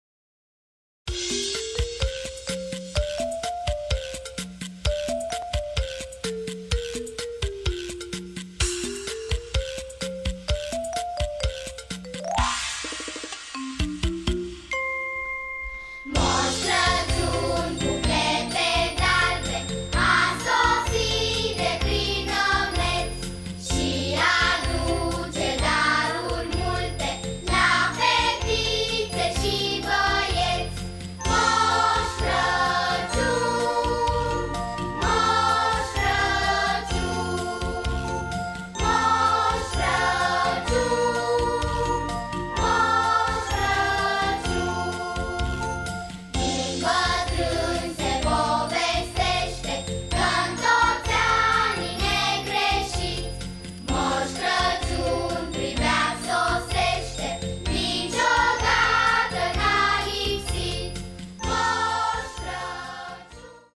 Mos Craciun a venit in Barcelona
Nu este zapada? Nu e nimic!
Bucuria copiilor este la fel de mare chiar daca multi dintre ei au vazut zapada doar in fotografii :)
L-au asteptat nerabdatori la Parohia Ortodoxa din Barcelona unde parintele Bunda a pregatit o sarbatoare fara seaman.
Corul barbatesc al parohiei a reinviat traditiile iar corul de copii si-a cantat bucuria reintalnirii cu Mos Craciun.